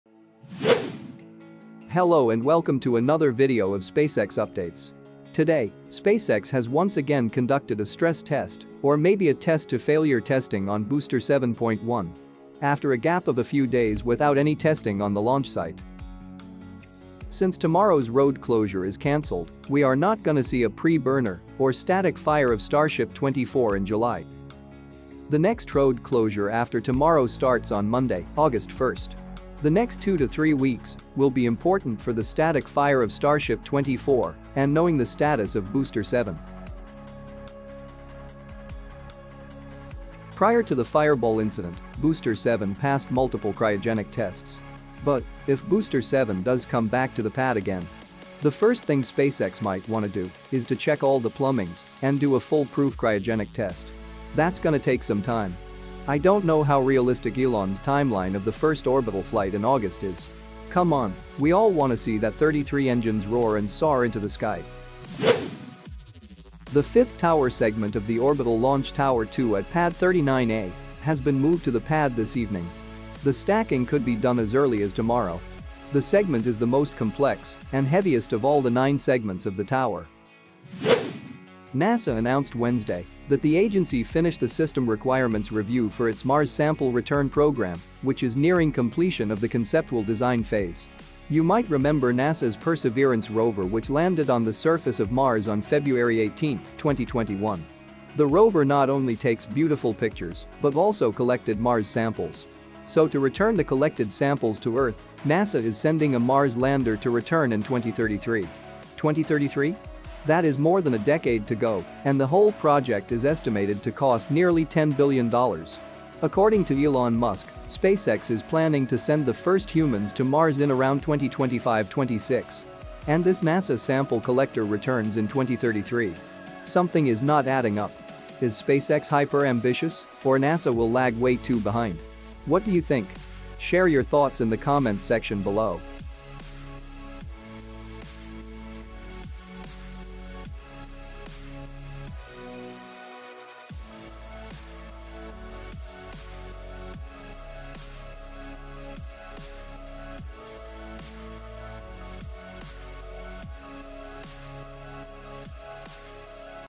Test to Failure on Booster 7.1, Tower Segment 5 Roll Out, Mars Sample Return Mission | SpaceX Update
SpaceX conducted a test to failure on Booster 7.1 by pressing under high pressure. Tower segment 5 of the Orbital Launch Tower at Pad 39A rolled out to the pad. NASA announced to bring Mars Sample to Earth in 2033.

Credit: Ocean Camera Space Corp., Labpadre, Starship Gazer, CnunezImages, NASA,SpaceX.